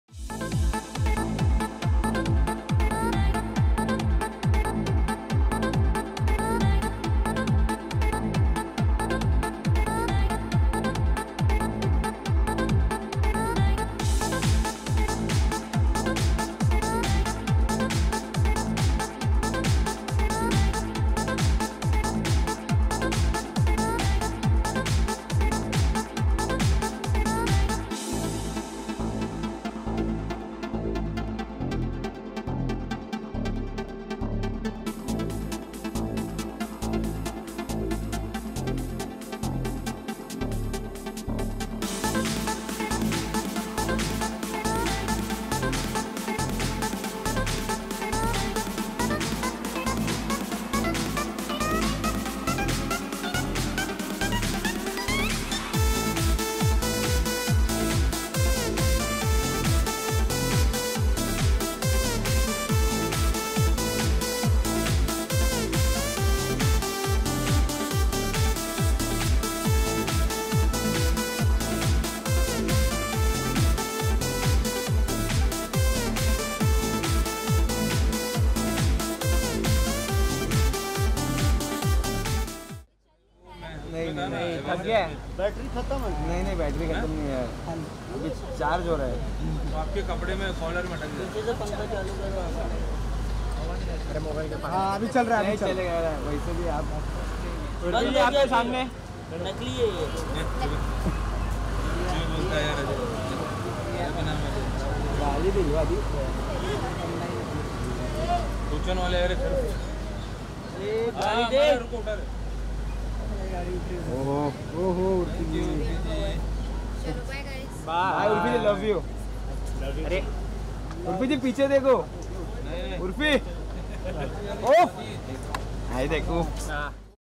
Is Urfi Javed's Latest Rotating Outfit Inspired From A Windmill?
Urfi Javed gets snapped in the suburbs in her unique look as always, which revolved and radiated full windmill vibes!